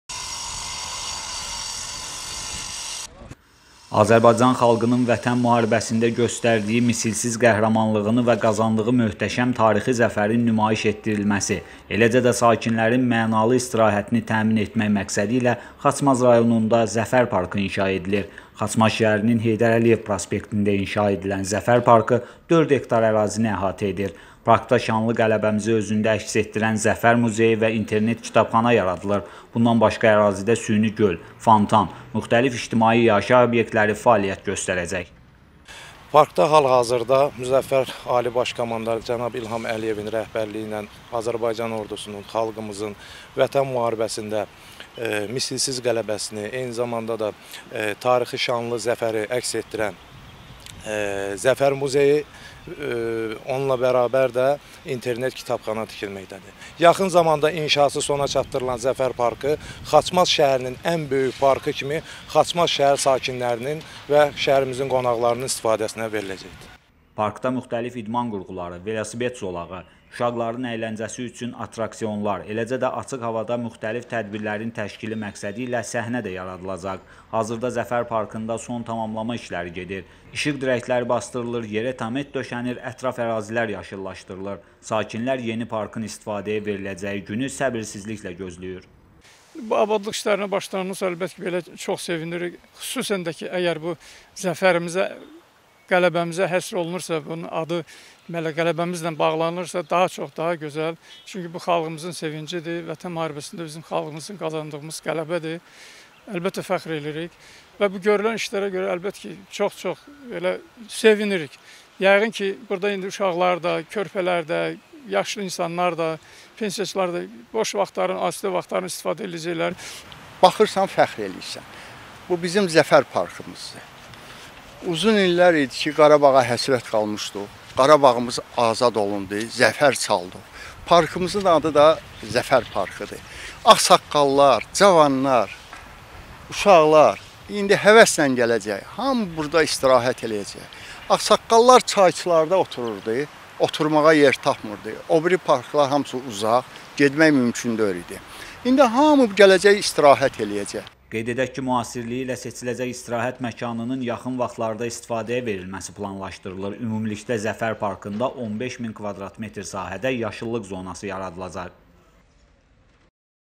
Xaçmazda Zəfər Parkı inşa olunur
Heydər Əliyev prospektində inşa edilən Zəfər Parkı 4 hektar ərazini əhatə edir